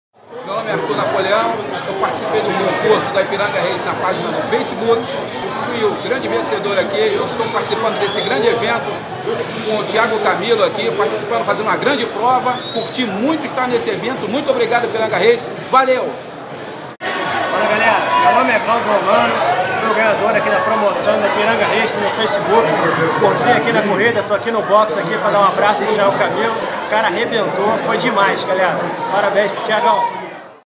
Vencedores do concurso Acelera comemoram o prêmio
Os vencedores do concurso cultural "Acelera", realizado na página do Ipiranga Racing no Facebook, comentam sobre a corrida da Stock Car no Rio de Janeiro